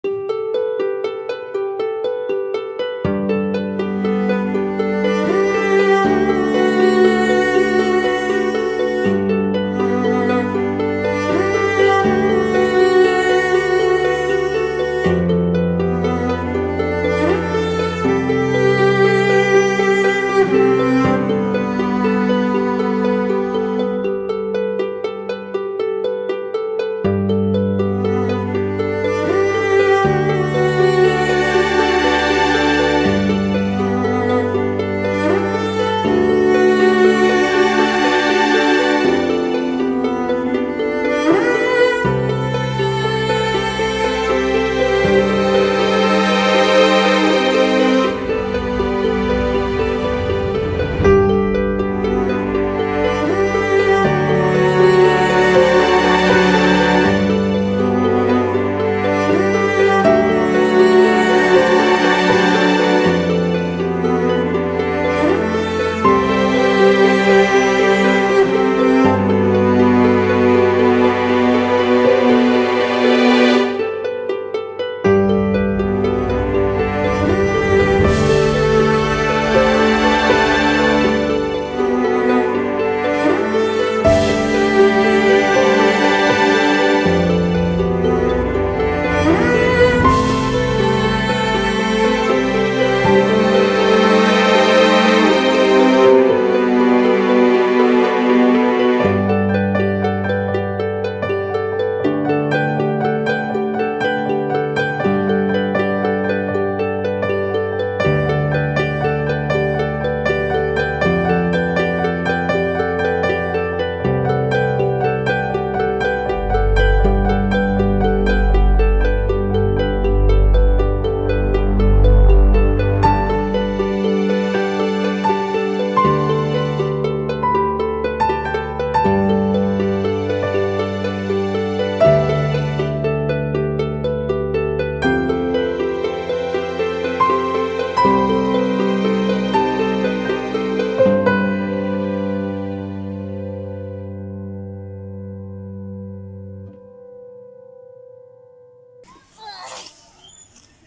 The Superman & A Cute Micky | Smart Kids playtime in Park | Kids Videos | Show For Kids
The Superman s are enjoying Swinging while a tired Micky is searching for a seat to take